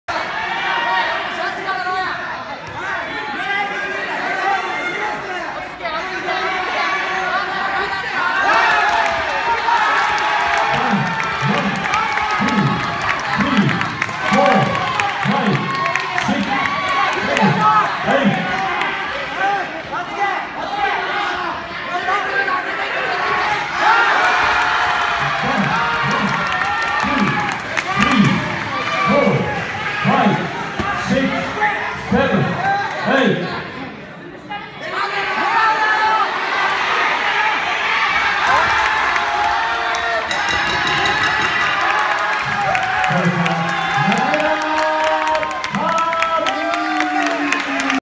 【苦戦した3ラウンド】中村寛選手プロ2戦目3RKO勝ち！《official》
2017/11/26 HOOST CUP KINGS OSAKA 2

オープニングファイト第一試合
中村寛 VS RYUYA

オープニングファイト第一試合目から3RKO勝利をおさめたが、いくら蹴っても立ち上がってくるRYUYA選手に、3ラウンド目まで苦戦させられた中村寛選手のプロ2戦目の3ラウンド目の映像。

Instagram.com/k_brosa
Twitter.com/k_brosa